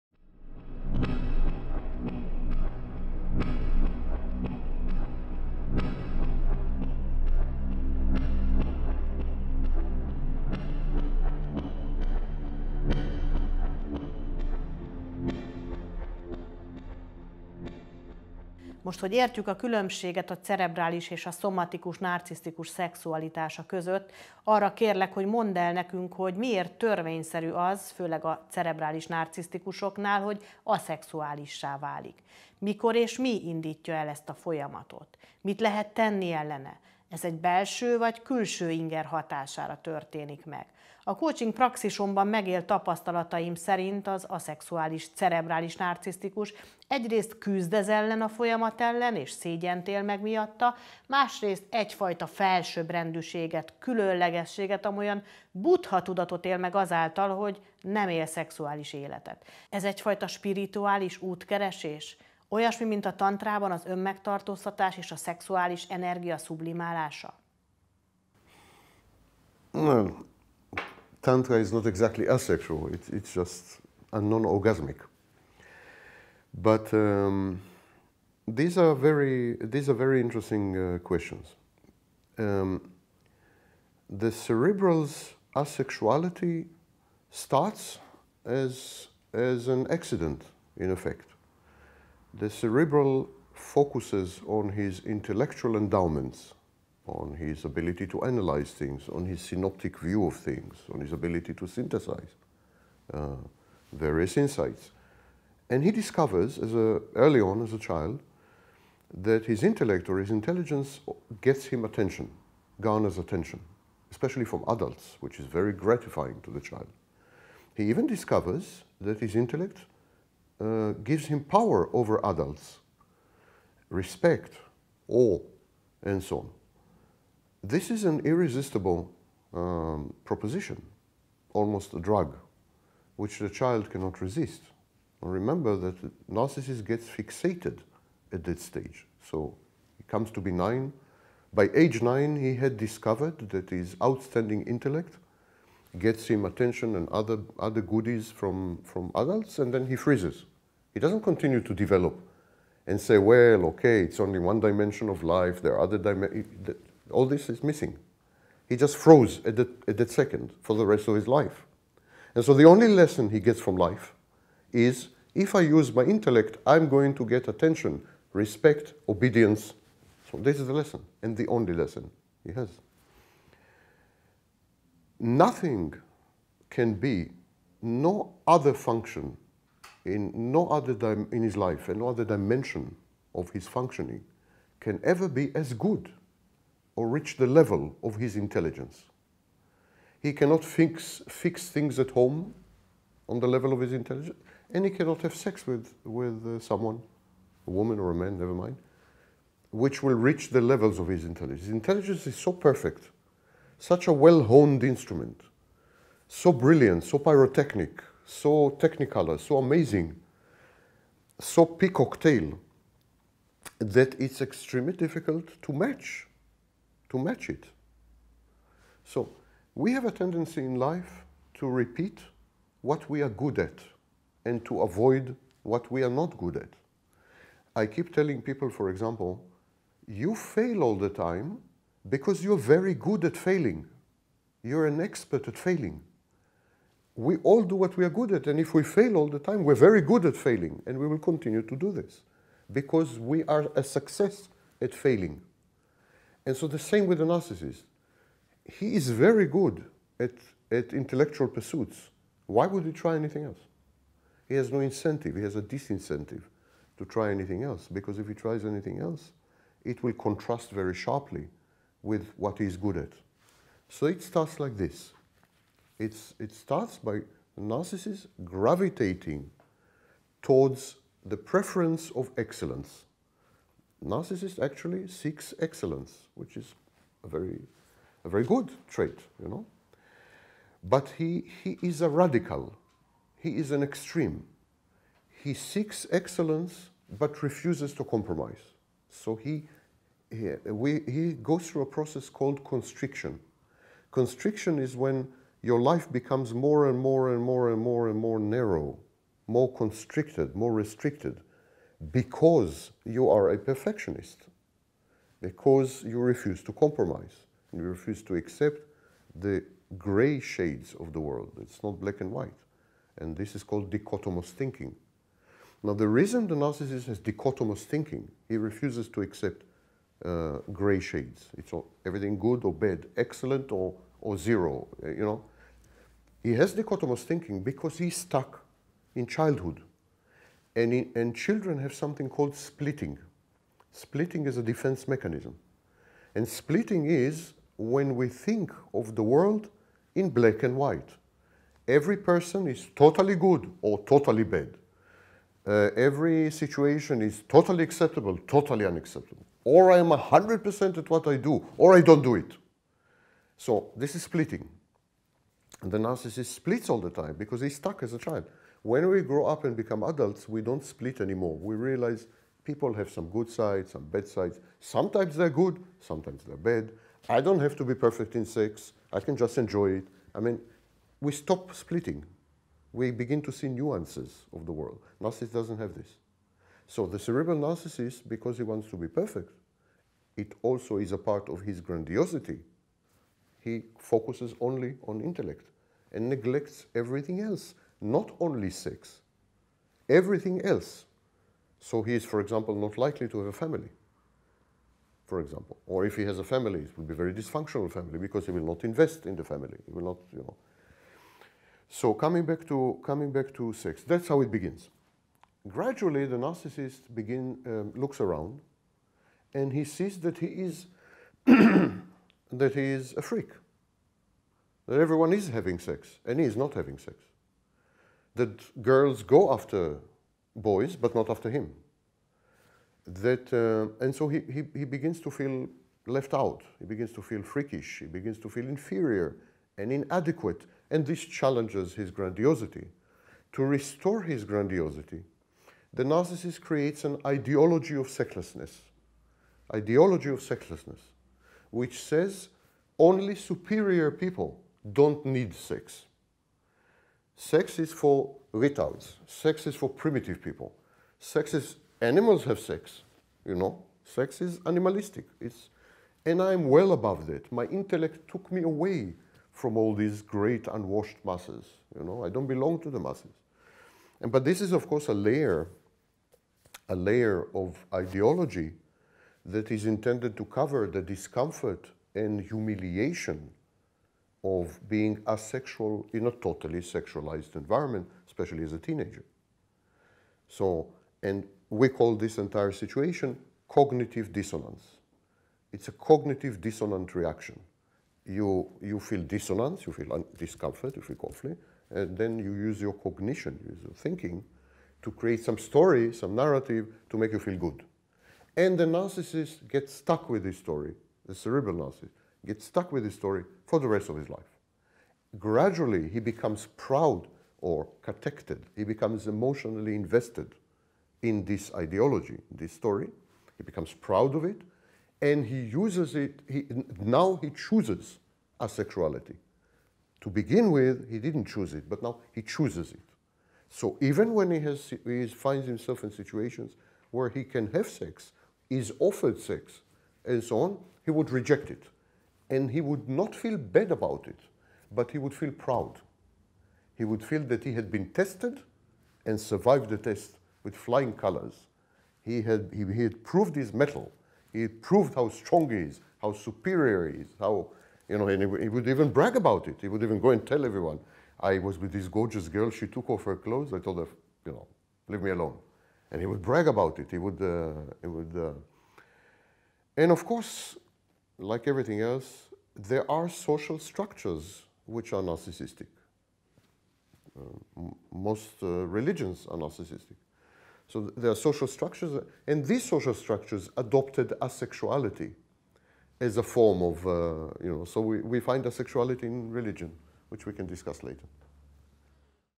Cerebral Narcissist's Proud Asexuality (ENGLISH responses)
The Narcissist's Psychosexuality: From Fantasy to Deviance and Beyond (ENGLISH responses) - Part 2 of 22

2. Now that we understand the difference between the sexuality of the cerebral and the somatic narcissist, please tell us why it is inevitable, especially for cerebral narcissists, that they become asexual? What does trigger this process and when? What can be done against it? Is this triggered by an internal or an external factor? Based on my experience in my coaching praxis, asexual cerebral narcissists try to fight this process and feel ashamed for it on one hand. But on the other hand, they feel superior and special by not having sex. It is kind of a “Buddha” state of mind. Is this some sort of spiritual quest like sexual abstinence and sublimation of sexual energy in tantra?